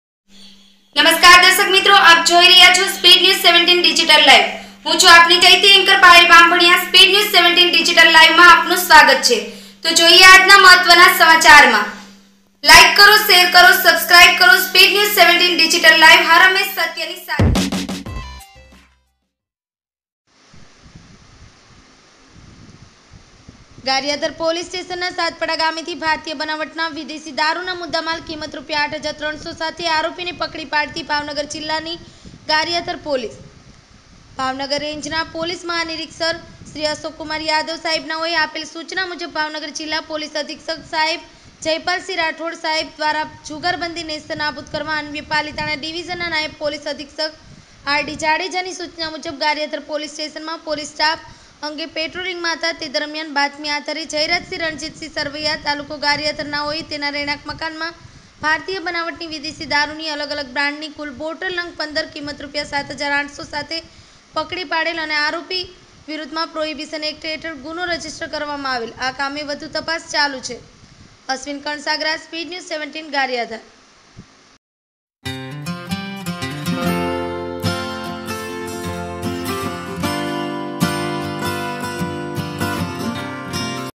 GARIYADHAR પોલીસ સ્ટેશનના સાતપડા ગામેથી ભારતીય બનાવટના વિદેશી દારૂ ના મુદામાલ કિ.રૂ.૮,૩૦૦/- સાથે..
ગારીયાધાર પોલીસ સ્ટેશનના સાતપડા ગામેથી  ભારતીય બનાવટના વિદેશી દારૂ ના મુદામાલ કિ.રૂ.૮,૩૦૦/- સાથે આરોપીને પકડી પાડતી ભાવનગર જીલ્લાની ગારીયાધાર પોલીસ
ભાવનગર રેન્જના પોલીસ મહાનીરીક્ષક સા. શ્રી અશોકકુમાર યાદવ સાહેબ નાઓએ આપેલ સુચના મુજબ ભાવનગર જીલ્લા પોલીસ અધિક્ષક સાહેબ શ્રી જયપાલસિંહ રાઠૌડ સાહેબ દ્વારા પ્રોહી./જુગાર બદી નેસ્તનાબુદ કરવા અન્વયે પાલીતાણા ડીવીઝનના નાયબ પોલીસ અધિક્ષક સા. શ્રી આર.ડી.જાડેજાની સુચના મુજબ ગારીયાધાર પોલીસ સ્ટેશનના પોલીસ સ્ટાફ પ્રોહી. અંગે પેટ્રોલીંગમા હતા તે દરમ્યાન બાતમી આધારે  જયરાજસિંહ રણજીતસિંહ સરવૈયા તા.ગારીયાધાર નાઓને તેના રહેણાક મકાનમા ભારતીય બનાવટની વિદેશી દારૂની અલગ અલગ બ્રાન્ડની કુલ બોટલ નંગ ૧૫ કિ.રૂ.૭૮૦૦/- સાથે પકડી પાડેલ  આરોપી વિરૂધ્ધમા પ્રોહીબીશન એકટ હેઠળ ગુન્હો રજી. કરાવવામા આવેલ આ કામે વધુ તપાસ ચાલુ છે.

 રિપોર્ટ અશ્વિન કણસાગરા ગારીયાધાર